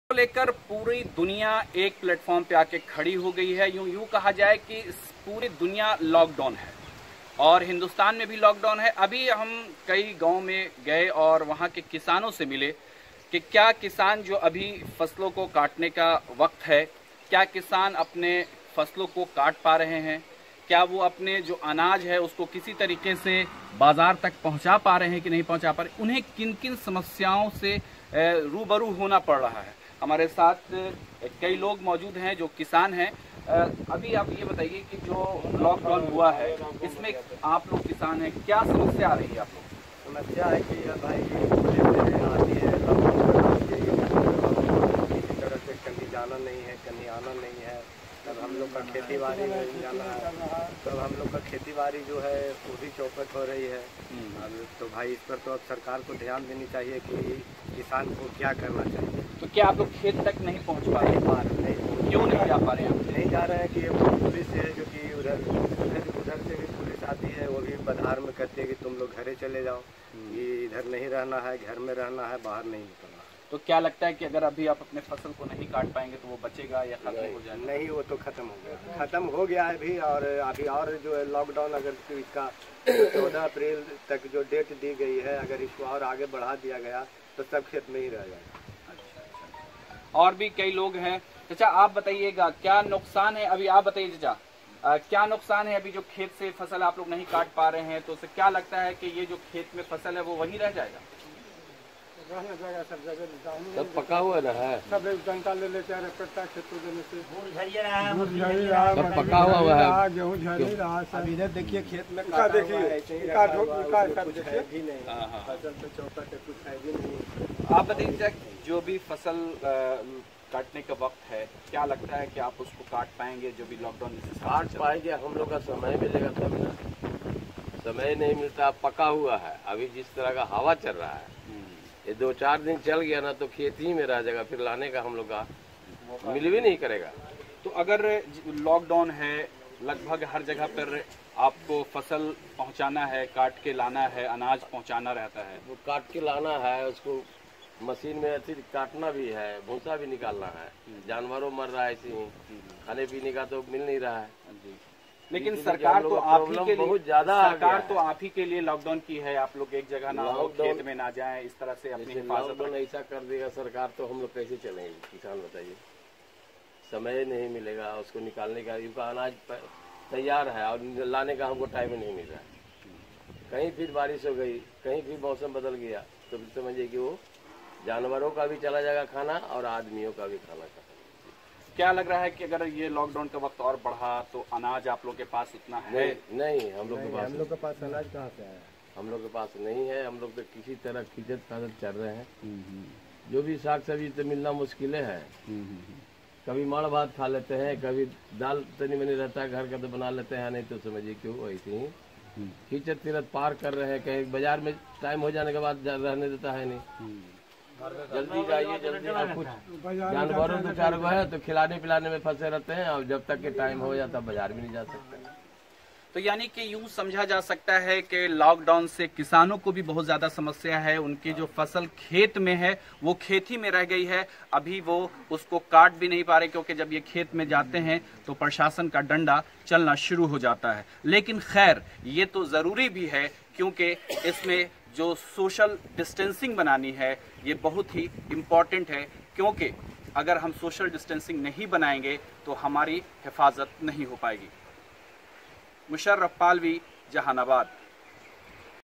जहानाबाद में लॉक डाउन के बीच किसान परेशान तो हैं ही लेकिन सरकार से बड़ी उम्मीद भी है , सुन लीजिए
जहानाबाद में लॉक डाउन के बीच किसान परेशान तो हैं ही लेकिन सरकार से बड़ी उम्मीद भी है , सुन लीजिए किसानों की समस्या